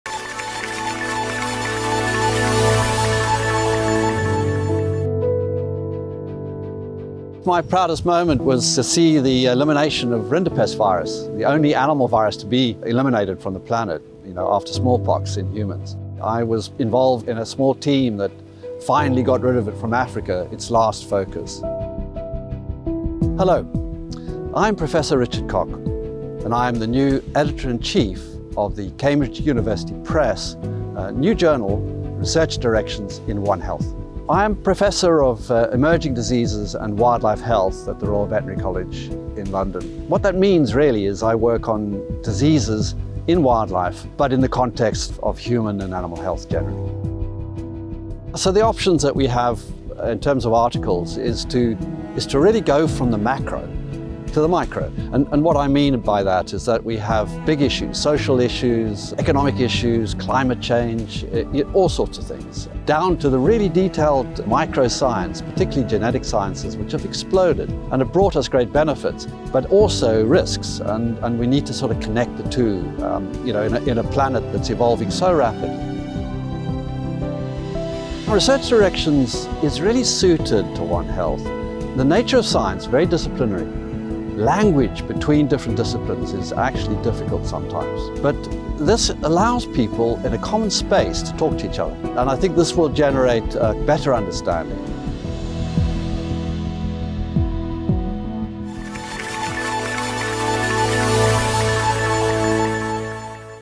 Meet Richard Kock: Editor-in-Chief of Research Directions One Health
Professor Richard Kock shares the proudest moment in his career so far, the different types of content Research Directions One Health is looking for and how it will help the research community.